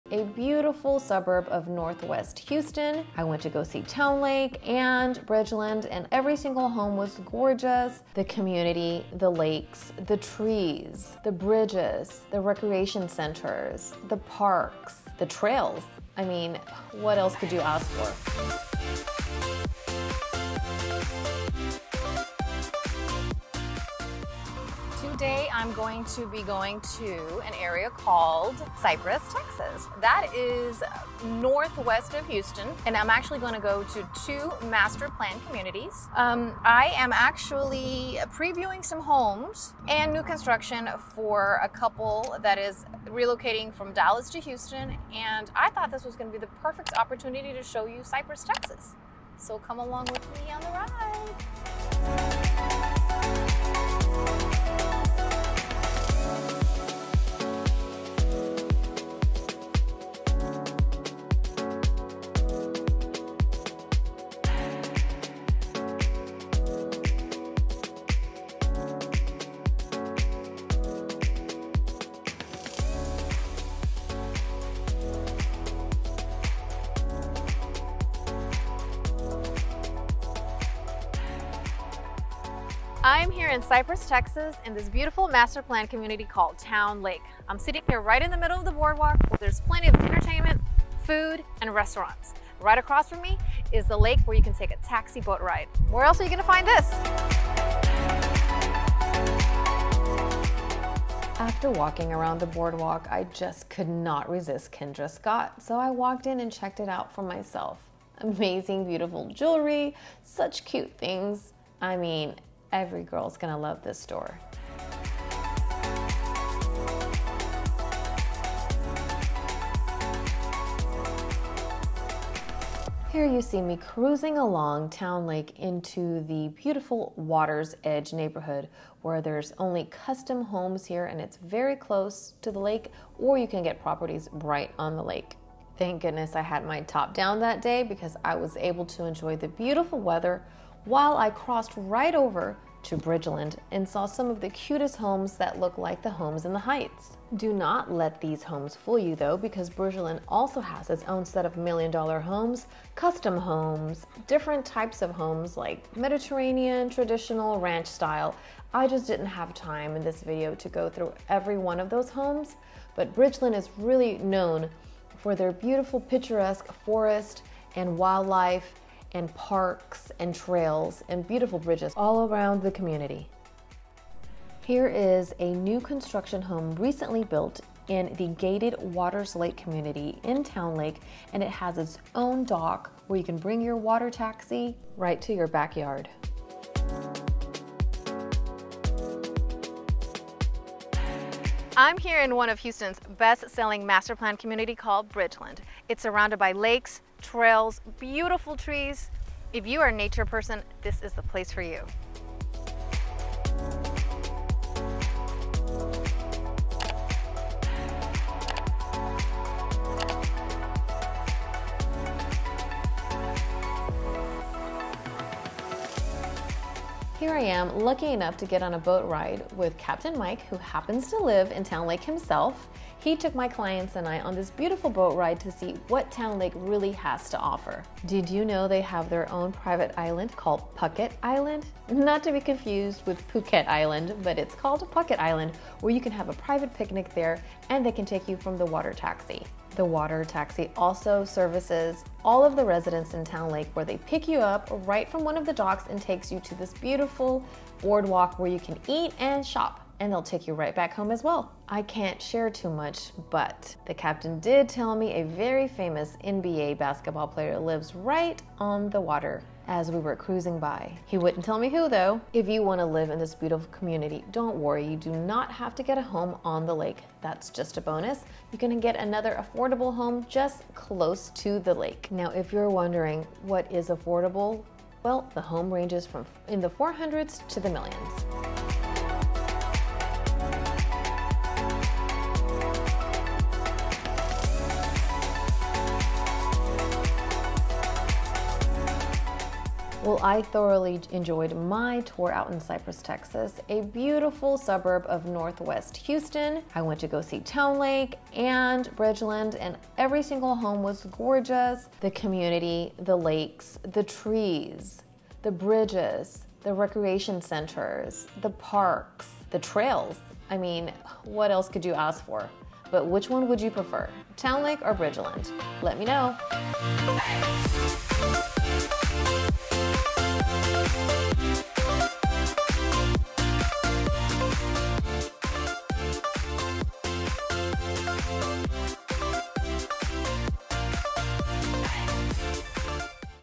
Discovering the Hidden Gems of Cypress, TX: Towne Lake and Bridgeland
Exploring Cypress, TX offers a multitude of great experiences, particularly in the charming neighborhoods of Towne Lake and Bridgeland. Nestled amidst beautiful lakeside houses and stunning natural scenery, these areas are home to a range of exciting activities and attractions.

Take a relaxing stroll around Towne Lake's Boardwalk, enjoy a day of kayaking on Bridgeland's many lakes, or simply soak in the serene surroundings. With its tranquil ambiance and picturesque vistas, this part of Cypress is a true delight to explore